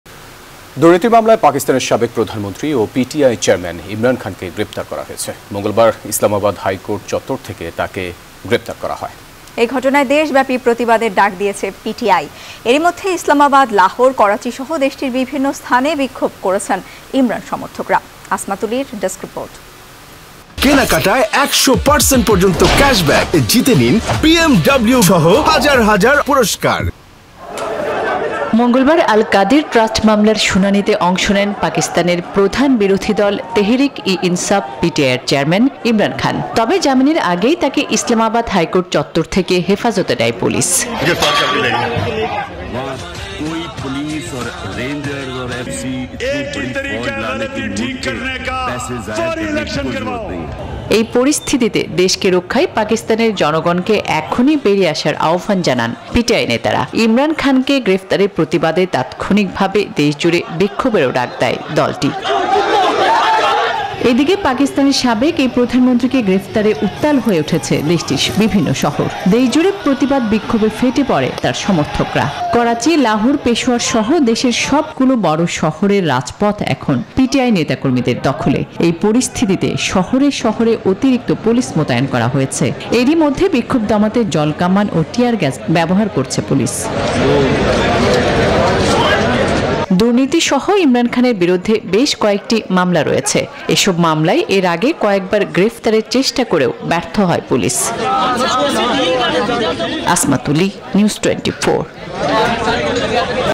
গ্রেপ্তার হলেন পাকিস্তানের সাবেক প্রধানমন্ত্রী ইমরান খান | Imran Khan | PTI | Pakistan | News24
মঙ্গলবার ইসলামাবাদ হাইকোর্ট চত্বর থেকে তাকে গ্রেপ্তার করা হয়। এ ঘটনায় দেশব্যাপী প্রতিবাদের ডাক দিয়েছে পিটিআই। এরই মধ্যে ইসলামাবাদ,  লাহোর, করাচিসহ দেশটির বিভিন্নস্থানে বিক্ষোভ করেছেন ইমরান সমর্থকরা
[Video Description/Information]

NEWS24 Team